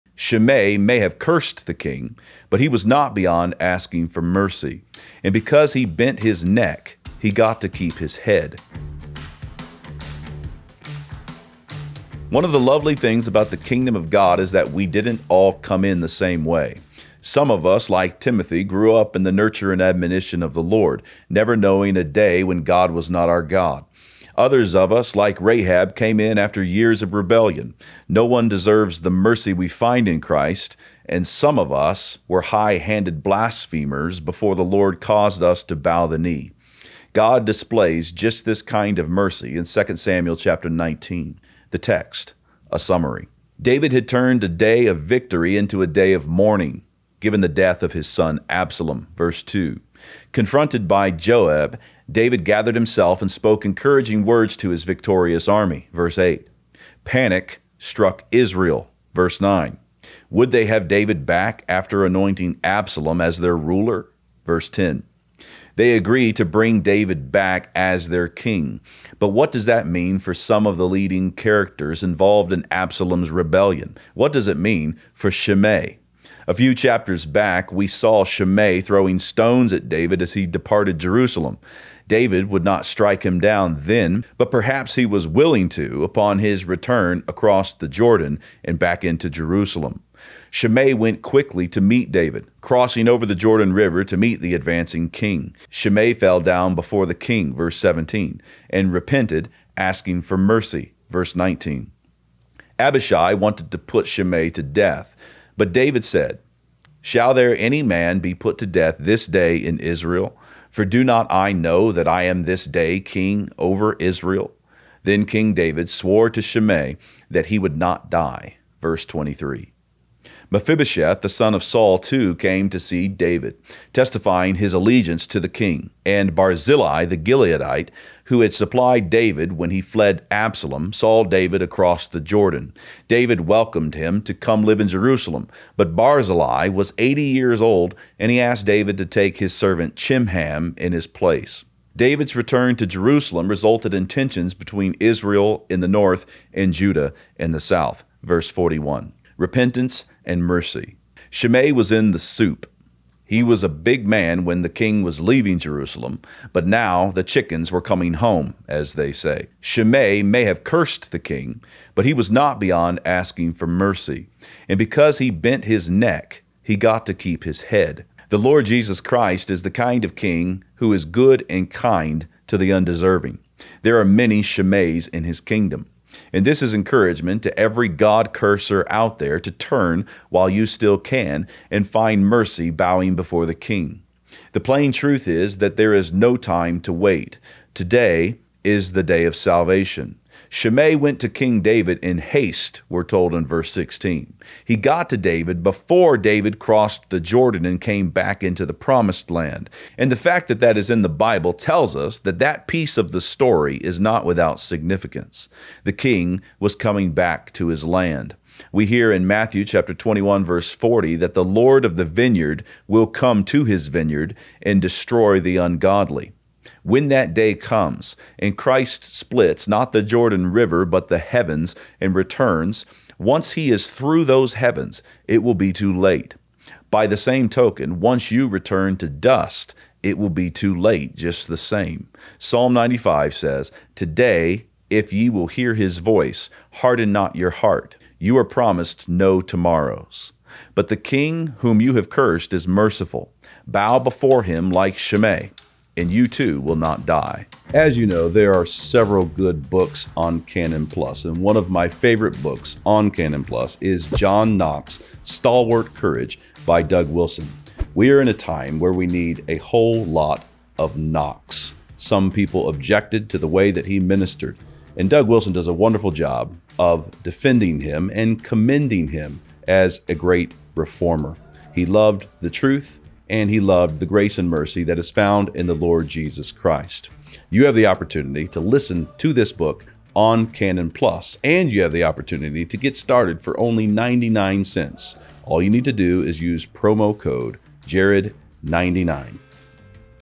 What Happens if You Curse God and Repent? | Jared Longshore
In this episode of Reformation & Revival, Pastor Jared Longshore discusses the question, "What happens if you curse God and repent?"